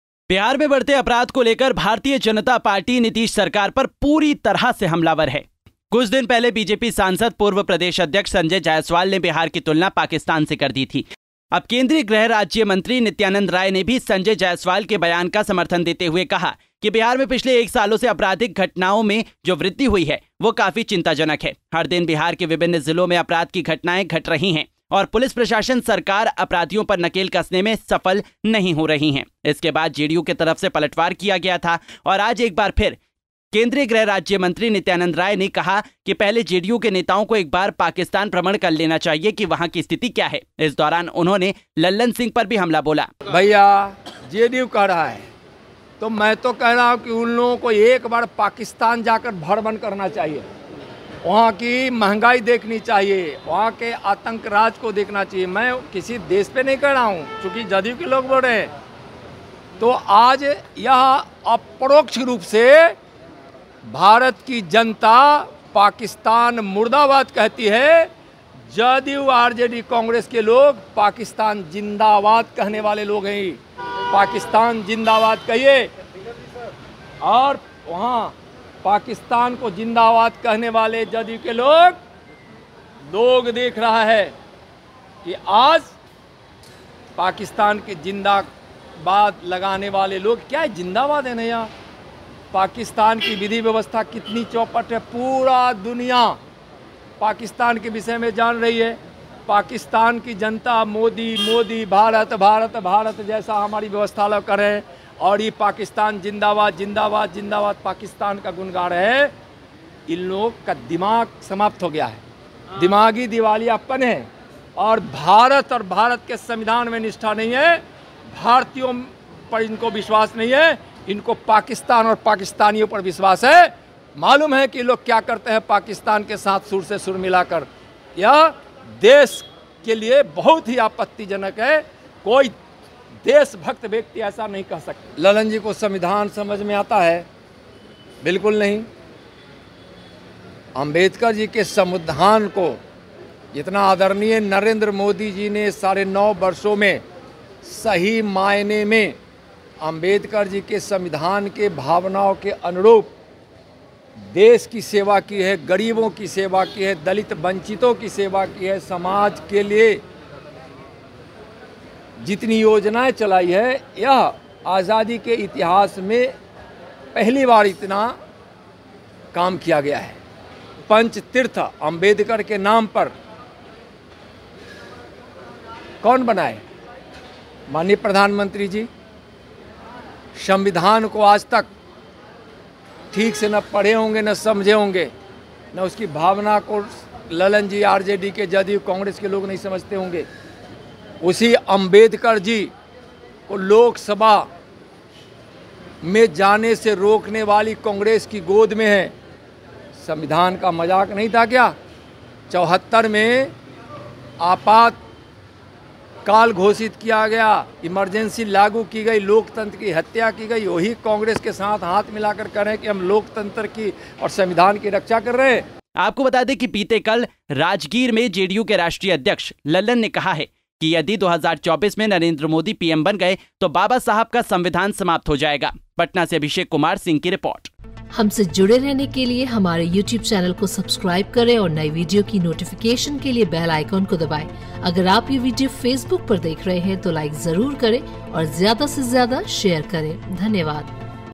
Nityanand Rai ने दी JDU नेताओं को Pakistan घूम कर आने की सलाह, कांग्रेस-राजद को लपेटा
केंद्रीय गृह राज्य मंत्री नित्यानंद राय(Nityanand Rai) ने भी संजय जायसवाल(Sanjay Jaiswal) के बयान का समर्थन देते हुए कहा कि... बिहार में पिछले 1 सालों से अपराधी घटनाओं में जो वृद्धि हुई है... वह काफी चिंताजनक है